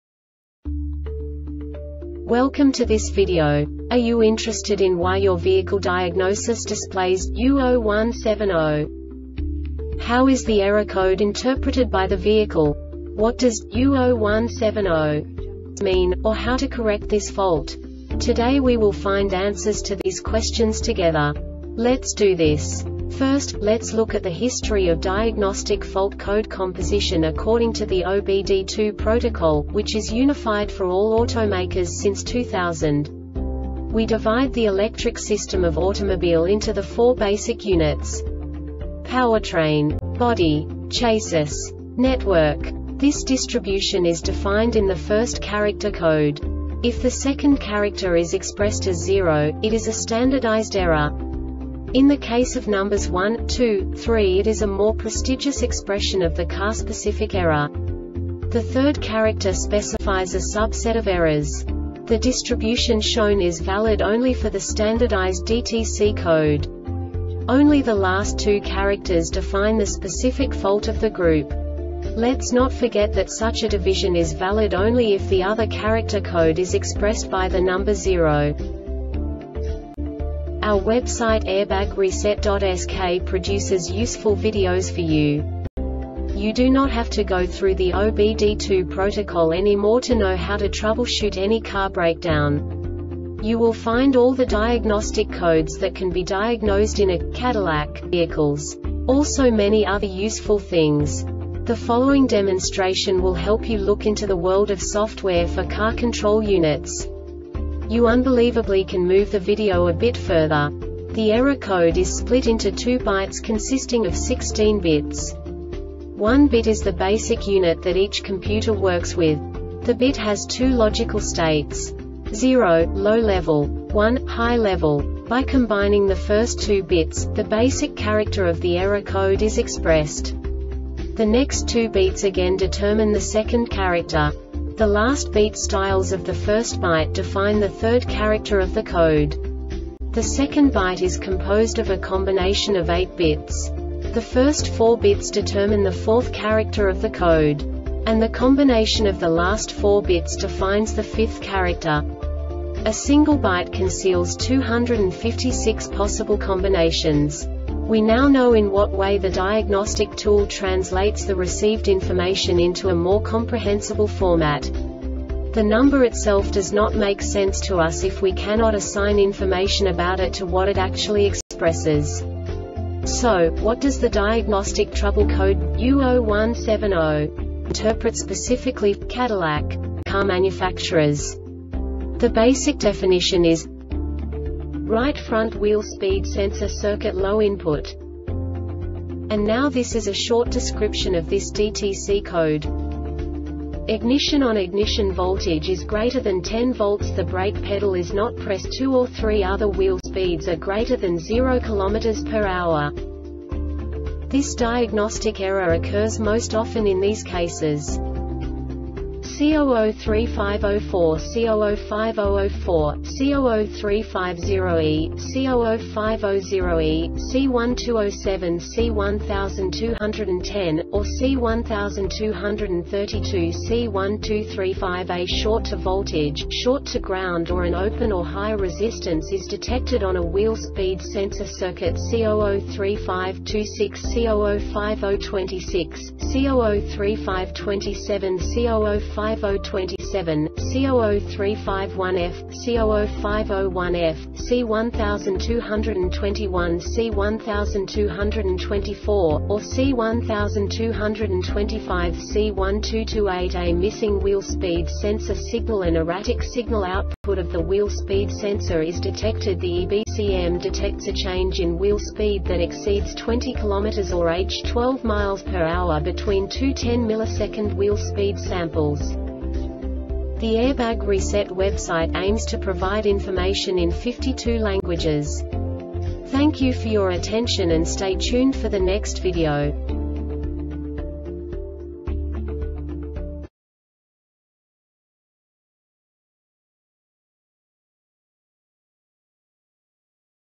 DTC cadillac U0170 Short Explanation
Contents:
0:21 Basic DTC analysis according to OBD2 protocol standard.
1:48 Insight into programming
2:58 A diagnostically understandable description of the Diagnostic Trouble Code
3:05 Basic error definition

Make:
Cadillac
Code:
U0170
Definition:
Right Front Wheel Speed Sensor Circuit Low Input
Description:
Cause:
C0035 04 - C0050 04, C0035 0E - C0050 0E,C1207 - C1210, or C1232 - C1235A short to voltage,short to ground or an open or high resistance is detected on a wheel speed sensor circuit.C0035 26 - C0050 26, C0035 27 - C0050 27, C0035 1F - C0050 1F,C1221 - C1224, or C1225 - C1228A missing wheel speed sensor signal.An erratic signal output of the wheel speed sensor is detected.The EBCM detects a change in wheel speed that exceeds 20 km or h (12 mph) between two 10 millisecond wheel speed samples.